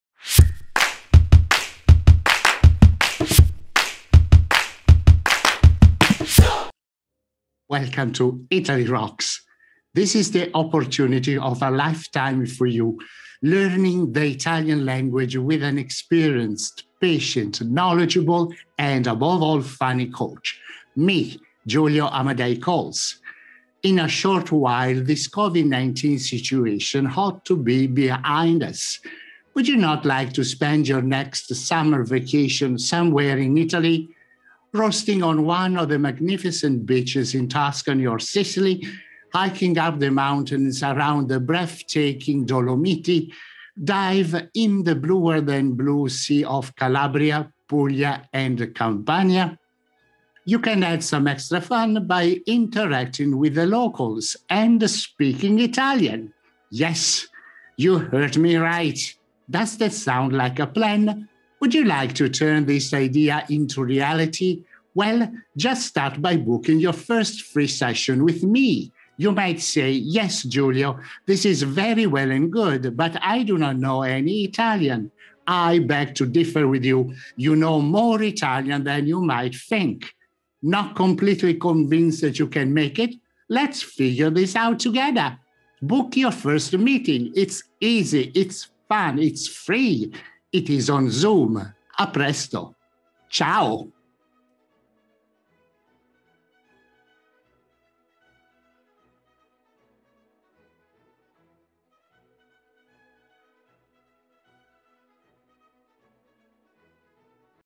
#24 ITALIAN CONVERSATION MEETINGS ON ZOOM.US
For all lovers of Italy and Italian culture, here is a golden opportunity to learn how to communicate effectively in Italian when vacationing there. These meetings are tailored to all those English-speaking people wanting to be capable of interacting with Italians in Italy. The approach is quite straightforward: transfer your native lexicon into sound, understandable Italian using the approach that ITALYROCKS has already implemented in many videos. Take advantage of this short-term opportunity: YOUR FIRST MEETING IS ON US!

ITALYROCKS logo animation by: BrunoUrzua